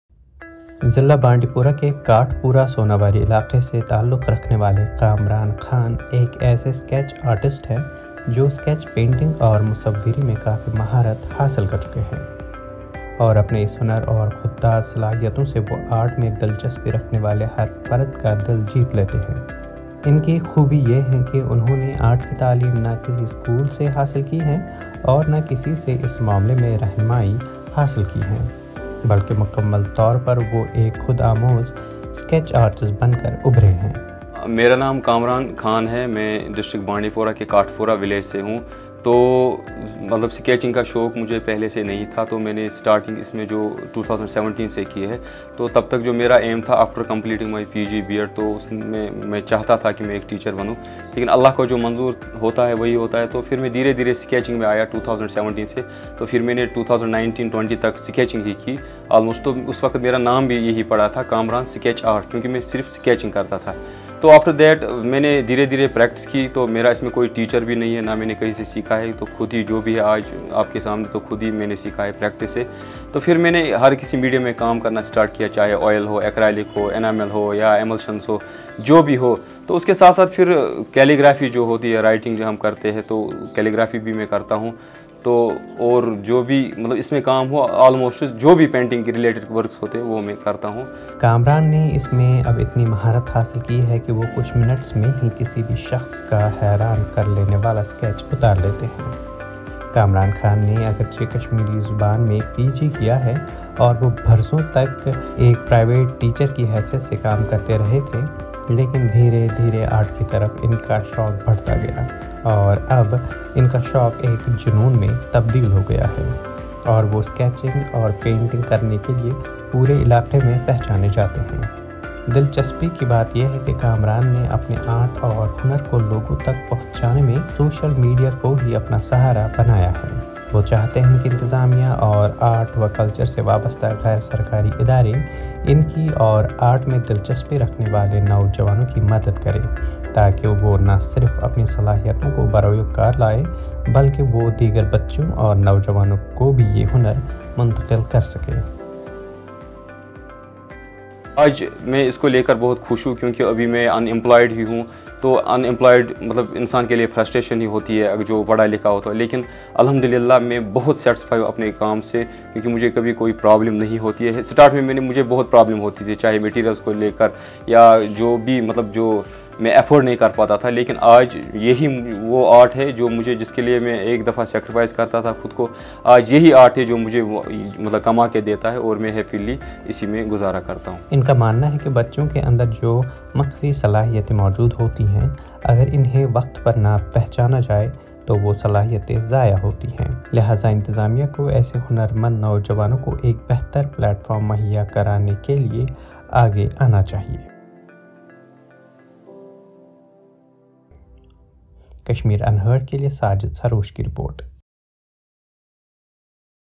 Self-Taught Art Sensation Kamran Khan Amazes Social Media with His Unique Masterpieces
Kamran Khan, widely known as Kamran Sketch Art, has taken the art world by storm with his extraordinary sketches and paintings shared on social media. A self-taught artist and painter, Kamran's talent has captured the admiration of countless art enthusiasts.

Unlike traditional art education, Kamran's passion for art was kindled by social media platforms, particularly YouTube, where he discovered his fascination for sketching. Armed with a mere pencil, he began crafting captivating art pieces that resonated with viewers.

Over time, Kamran's skills flourished, transforming him into a distinguished calligrapher and sketch artist in Bandipora. His incredible artworks have garnered widespread acclaim, leading him to showcase his talents in various exhibitions throughout Jammu and Kashmir.

Notably, Kamran's artistic journey runs parallel to his role as a teacher. Even with his professional commitments, he continues to win the hearts of people on social media with his continuous stream of unique and awe-inspiring creations.

Community Correspondent Sajid Sarosh reports from Bandipora, Kashmir) for Video Volunteers.  Learn more about Sajid and see other videos of his/hers